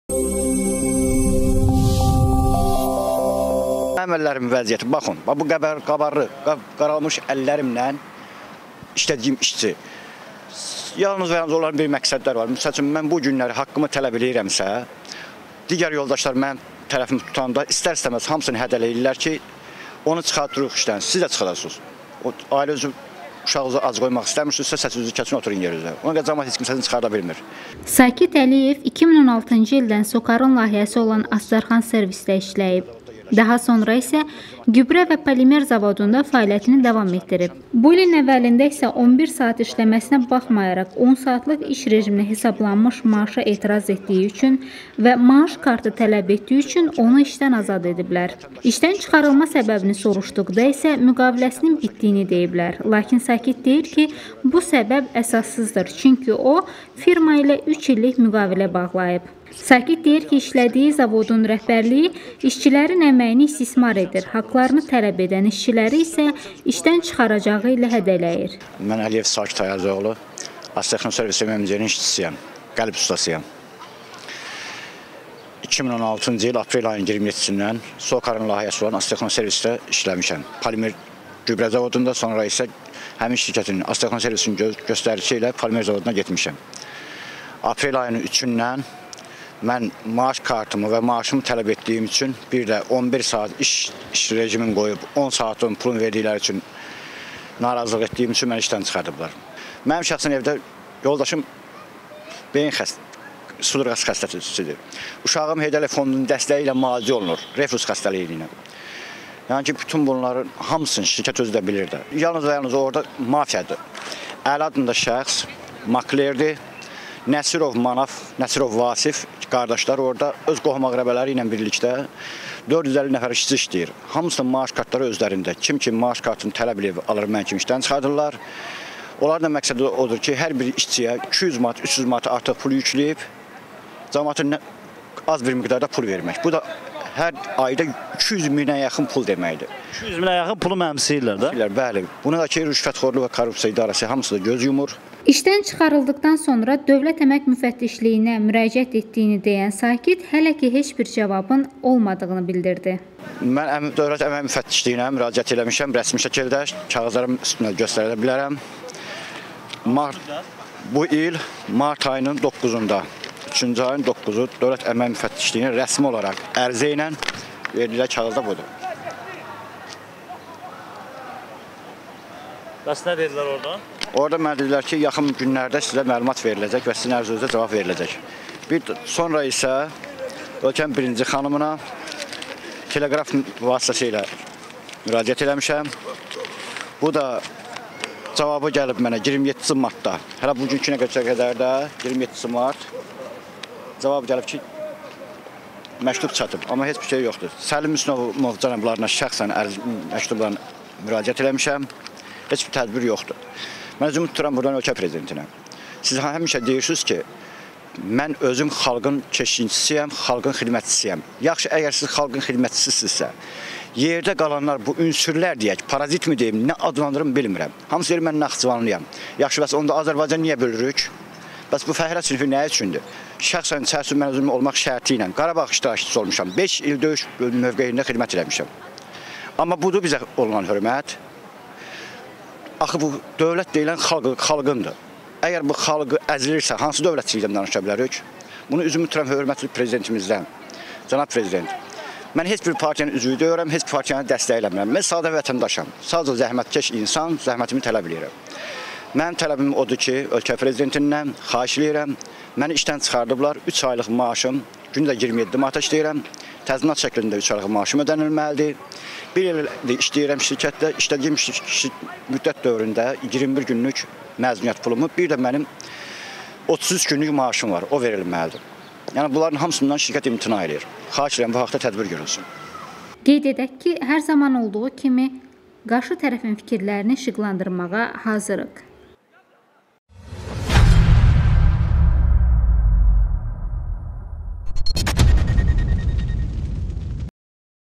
Maaşını və maaş kartını tələb etdiyi üçün vətəndaş işdən qovuldu
Sakit Əliyev 2016-cı ildən Socarın layihəsi olan "Astarxanservice"də işləyib.Daha sonra gübrə və polimer zavodunda fəaliyyətini davam etdirib.Bu ilin əvvəlində isə 11 saat işləməsinə baxmayaraq 10 saatlıq iş rejiminə hesablanmış maaşa etiraz etdiyi üçün və maaş kartını tələb etdiyi üçün onu işdən azad ediblər.